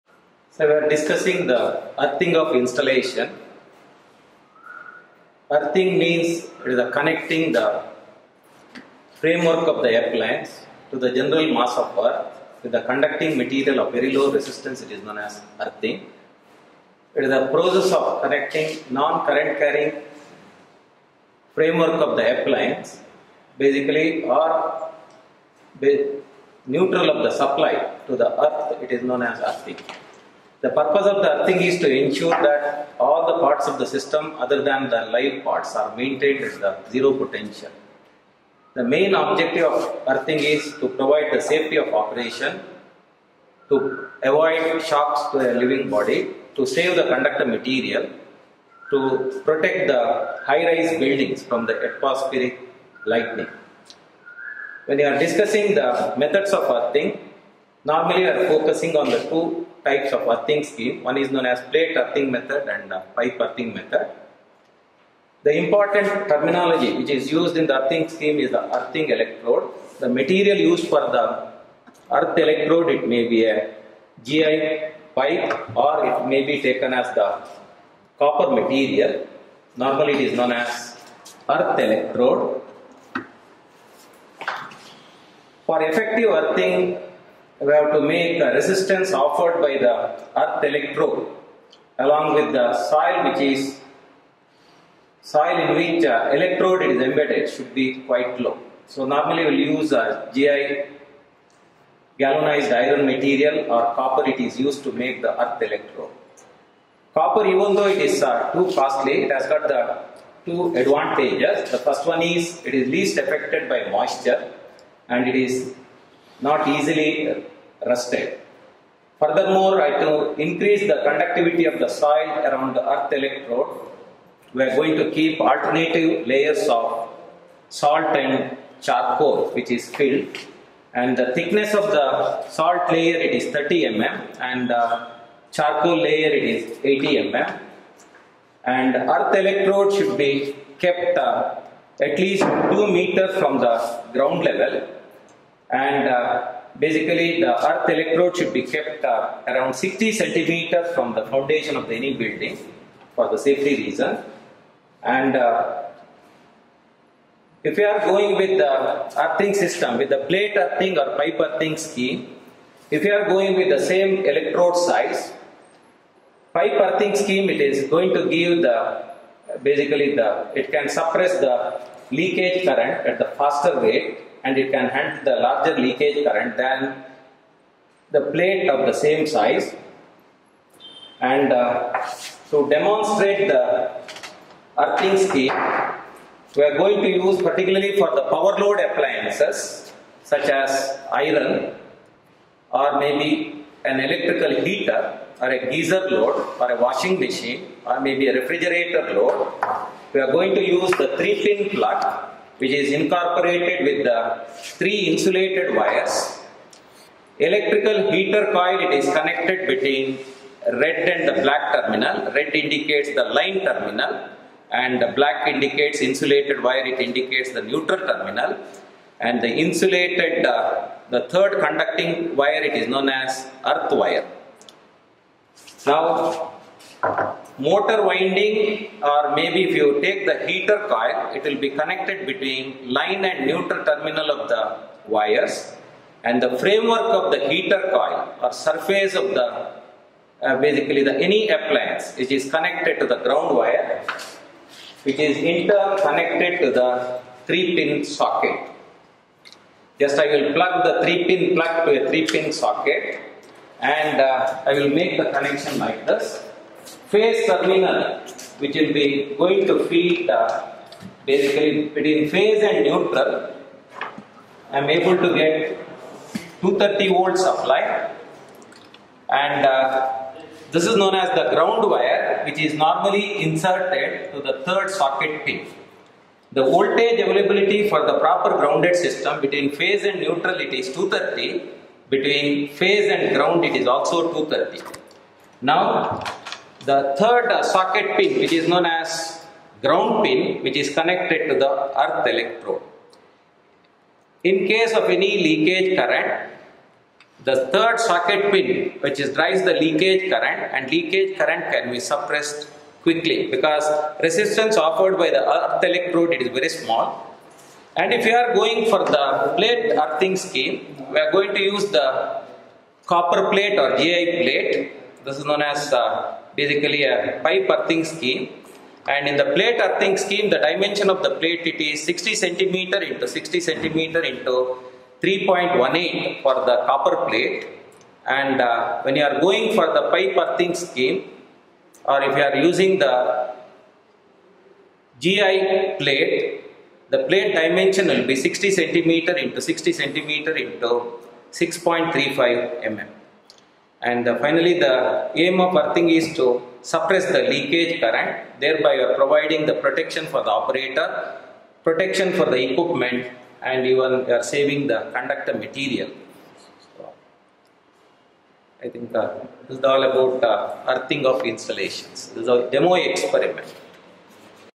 Experiment on safety devices & Earthing Demo
Aim: Experiment  on safety devices &Earthing (Demo)

Apparatus:  MCB ,Pipe earthing& Plate earthing